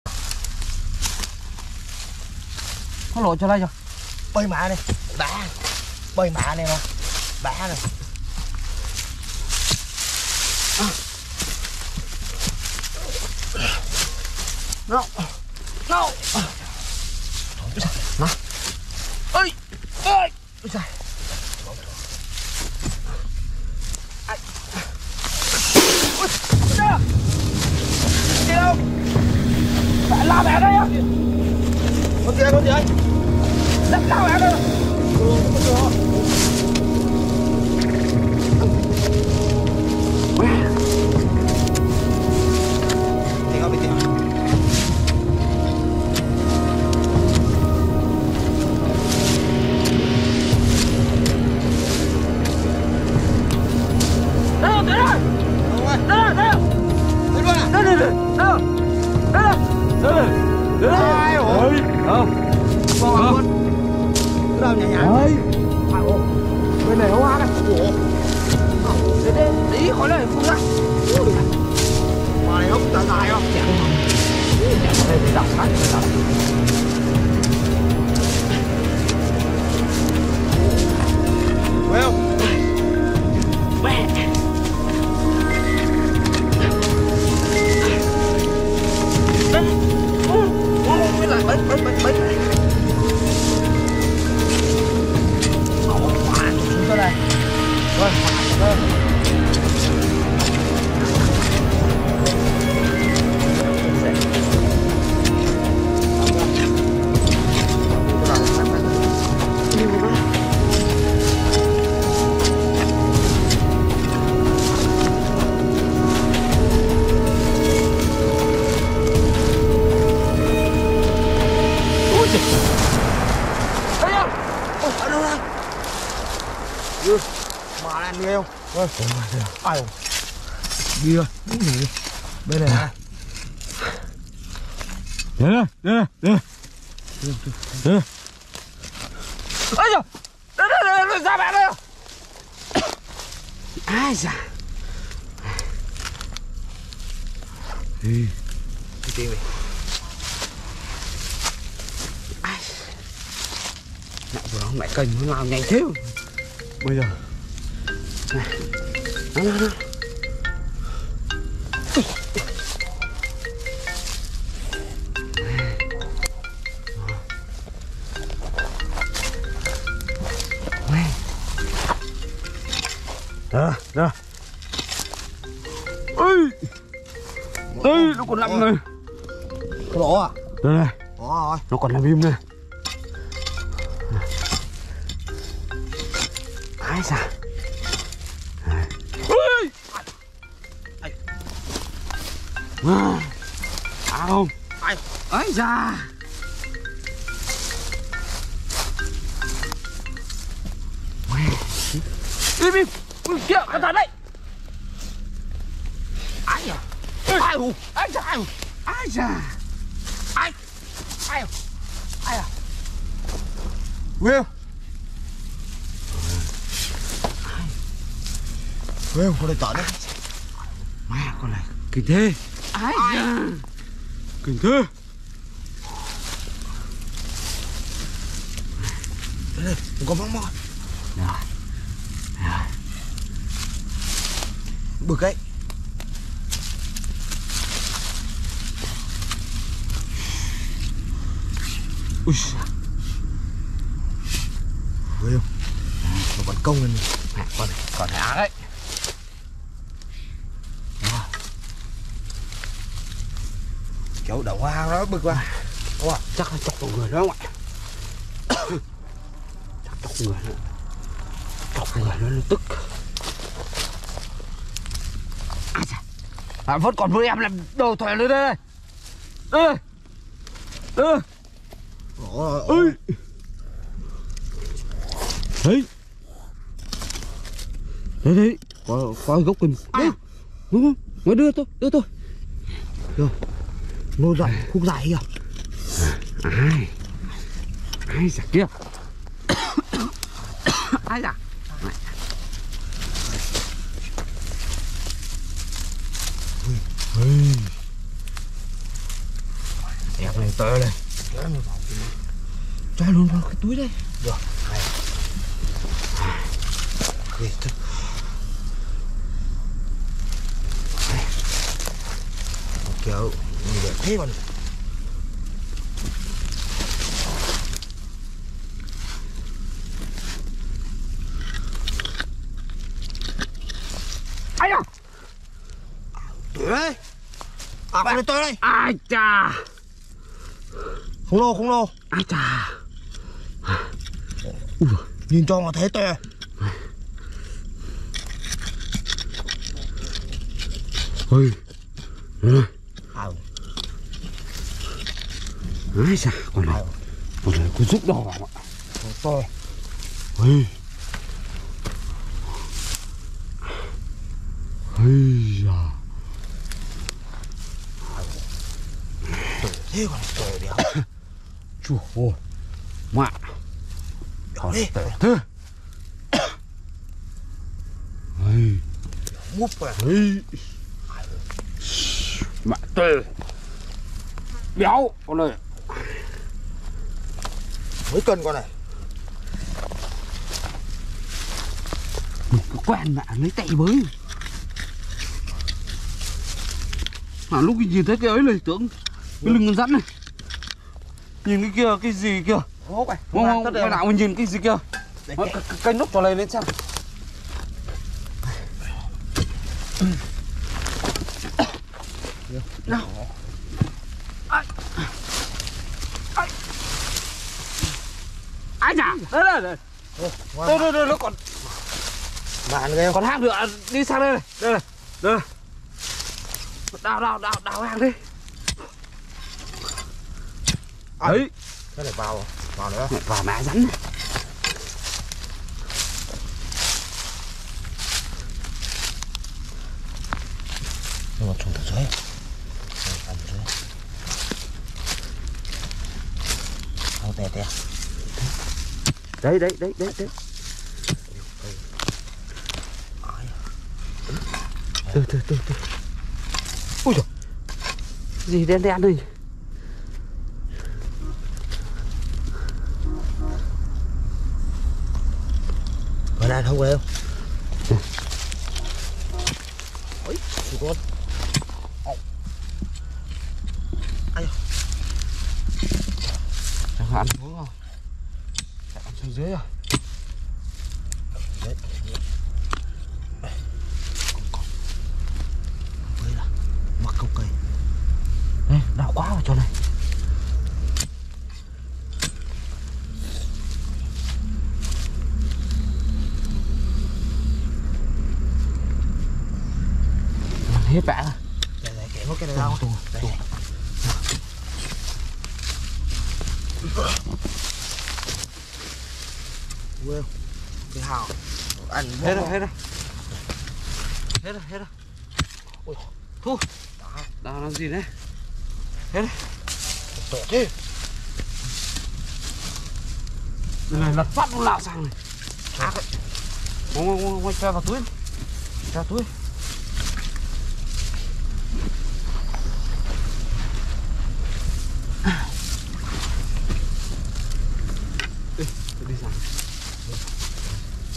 The terrifying moment of two hunters when confronting a herd of ferocious king cobras in the forest;
The actions in the video are performed by experienced people.
Ensure the safety of actors, including people and animals.
should not be followed in any way.